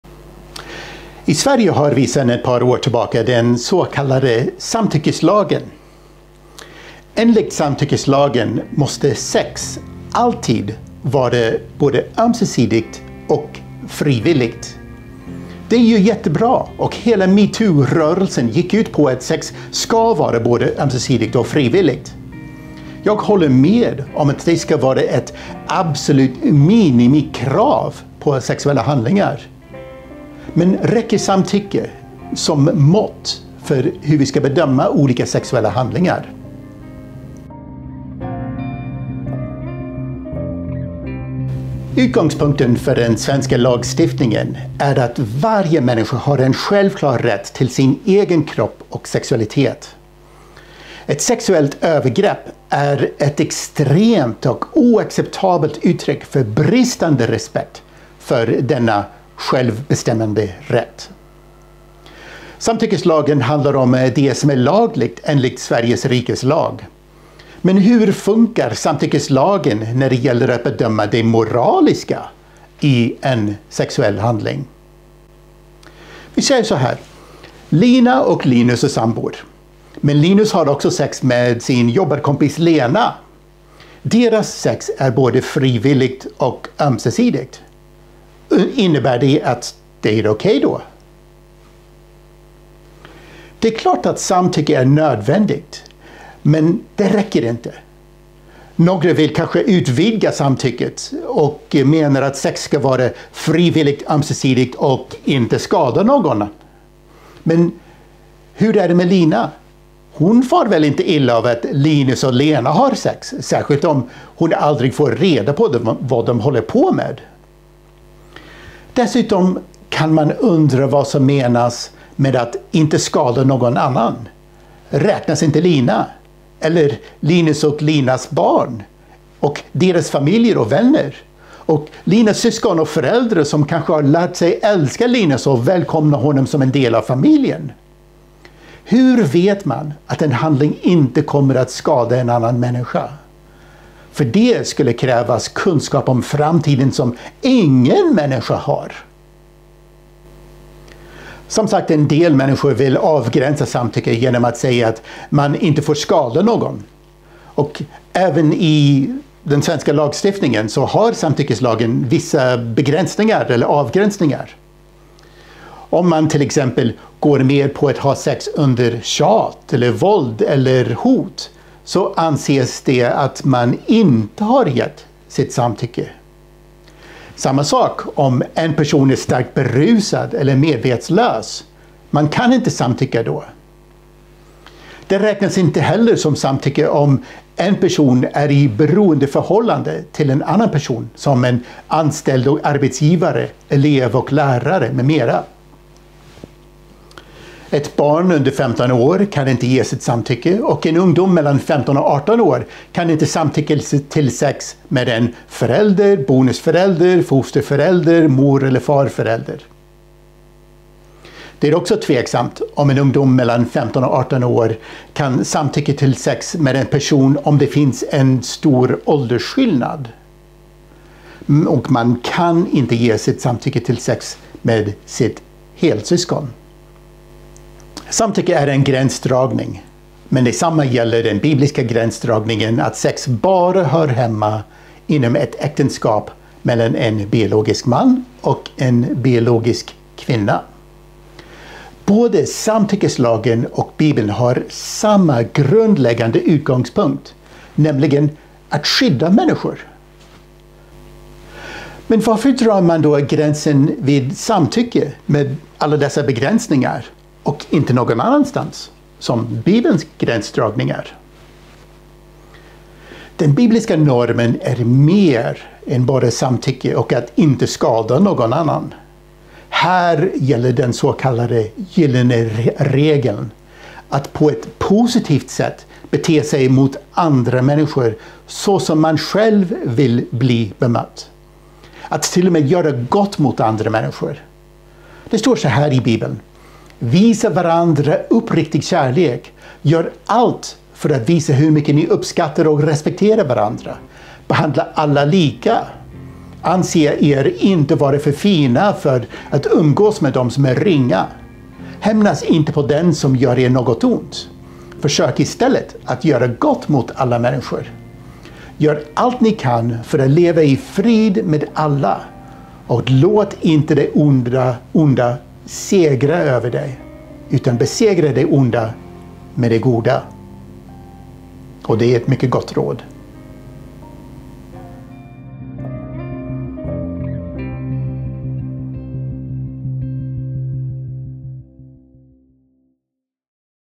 Samtyckeslagen
Dr Ray Baker, författare till boken "Det där med sex" förklarar den svenska Samtyckeslagen med dess begränsningar.

Läs mer om samtyckeslagen (och mycket annat) i boken "Det där med sex". Finns hos bokhandlare online och på